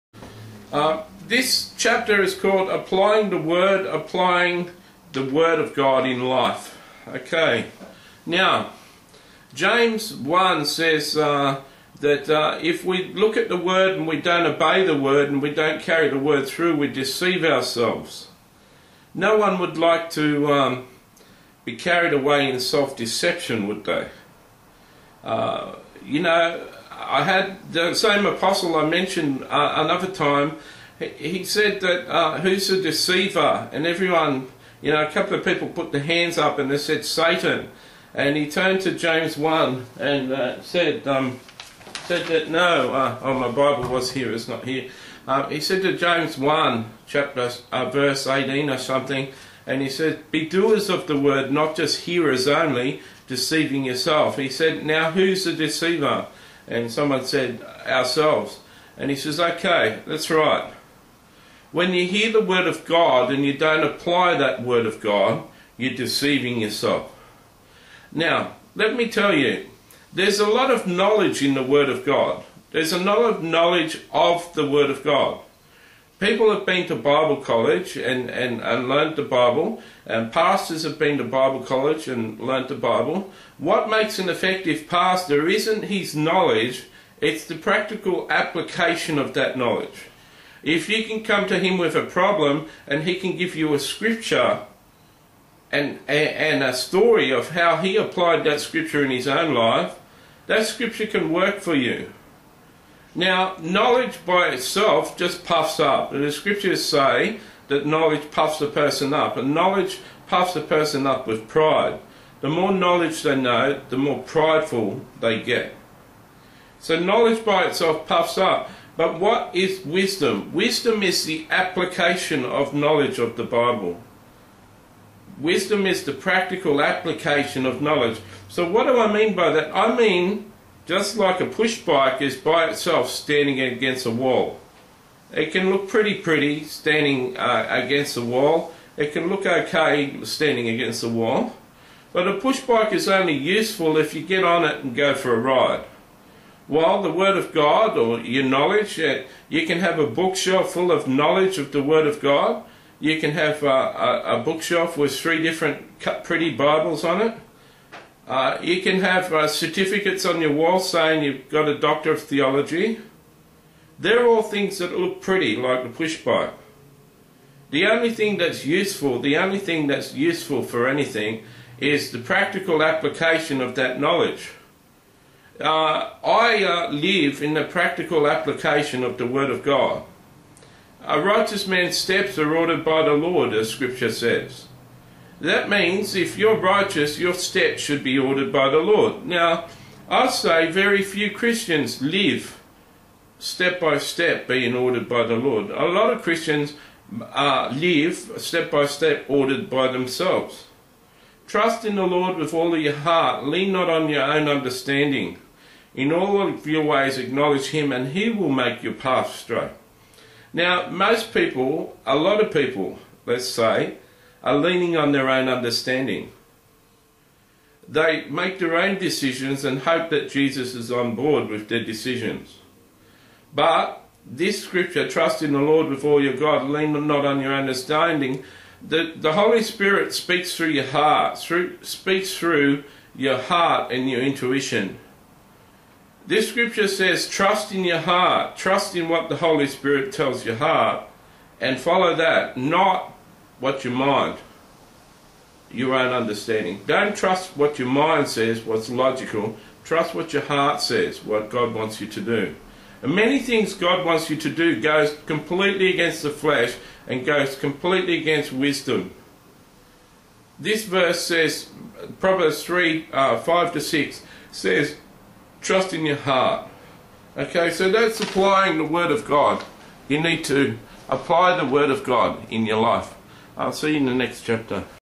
Applying the Word in your Life
you can find out more about Matthew Robert Payne.
Applying the Word in your Life is a video all about the role of the Word of God in your life and how to apply it for success